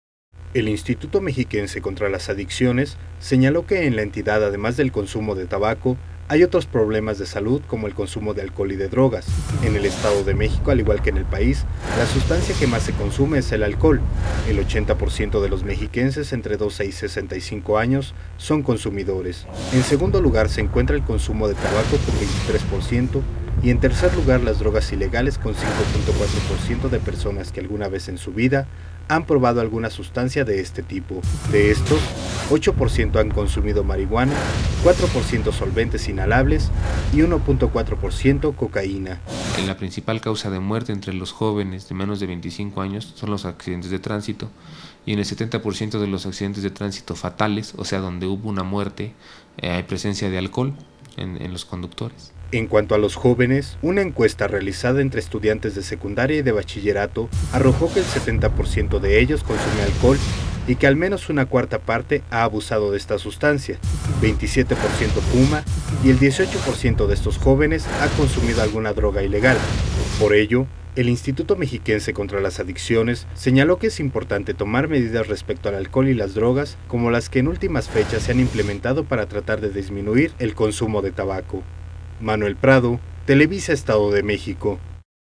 TABACO
El tabaco es una de las tres adiciones más frecuentes en el estado de México.